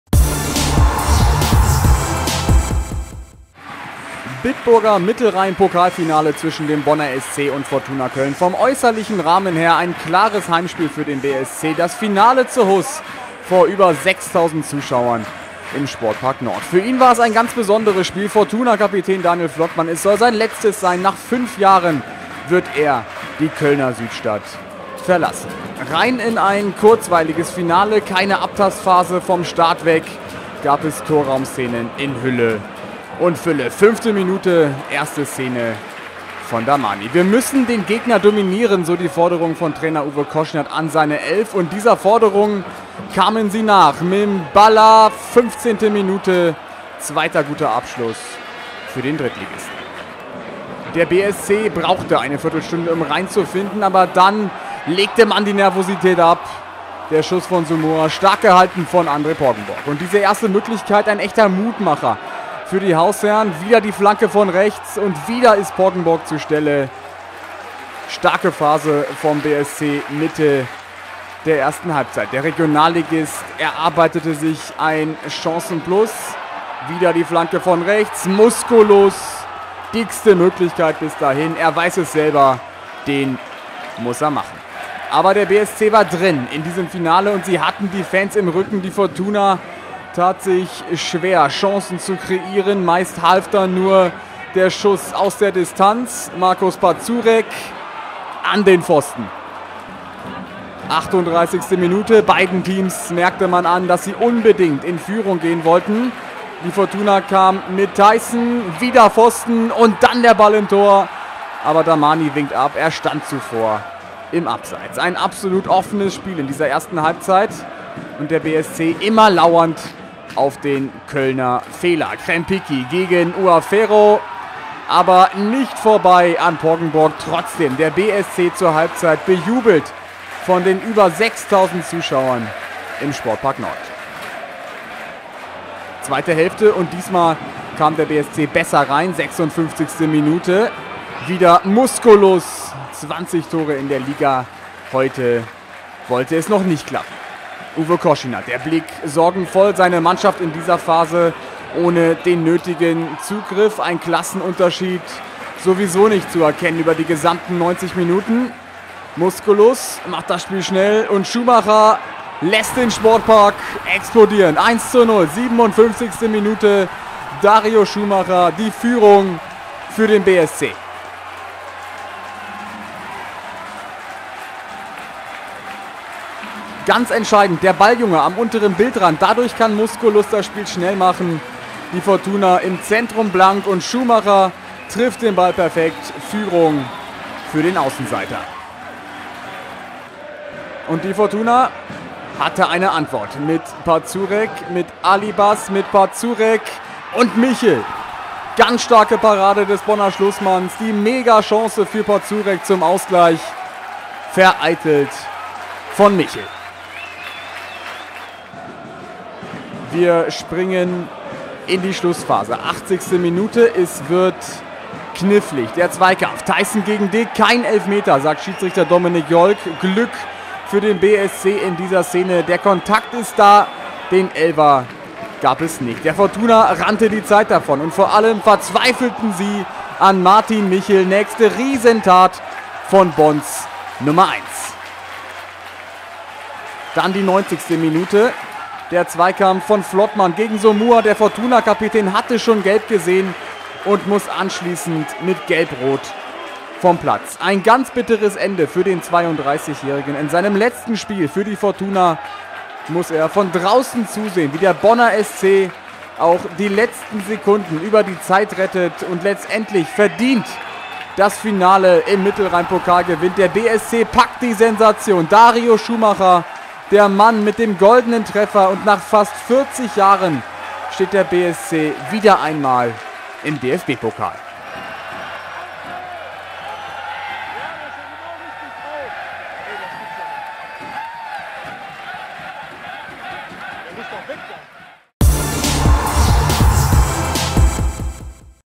SC Fortuna Köln - Bonner SC (Finale, Pokal der 1. Herren 2016/2017) - Spielbericht
Finale (Pokal der 1. Herren 2016/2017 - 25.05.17)

SC Fortuna Köln - Bonner SC

Tore:
0:1 Dario Schumacher

Aufstellung SC Fortuna Köln:
1 Andre Poggenburg, 3 Daniel Flottmann, 4 Bone Uaferro, 5 Markus Pazurek, 7 Michael Kessel, 10 Cauly Oliveira Souza, 14 Cimo Röcker, 15 Christopher Theisen, 27 Ebewa-Yam Mimbala, 28 lars Bender, 30 Hamdi Dahmani

Einwechselspieler:
38 Selcuk Alibaz, 11 Kai Bösing, 21 Kai Burger

Trainer:
Uwe Koschinat

Aufstellung Bonner SC:
1 Martin Michel, 2 Adis Omerbasic, 4 Mario Weber, 5 Andreas Dick, 7 Lucas Musculus, 9 Marcel Kaiser, 13 Connor Krempicki, 15 Kris Fillinger, 16 Ugur Dündar, 17 Daniel Somuah, 20 Dario Schumacher

Einwechselspieler:
14 Ricardo Retterath, 22 Günter Mabanza, 8 Tim Lünenbach

Trainer:
Daniel Zillken